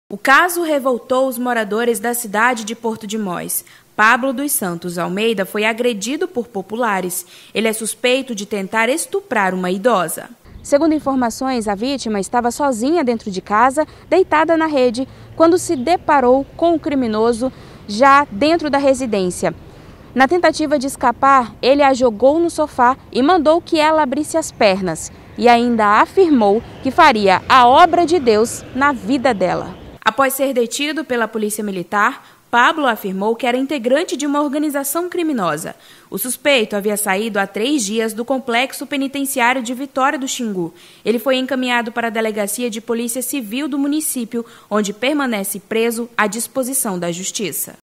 HOMEM TENTOU ABUSAR DE IDOSA ALEGANDO QUE ESTARIA FAZENDO A "OBRA DE DEUS"
Durante a tentativa o acusado teria dito que era evangelista e que pediu para mulher abrir as pernas para ele “fazer a obra de Deus” na vida dela, segundo relato da vítima.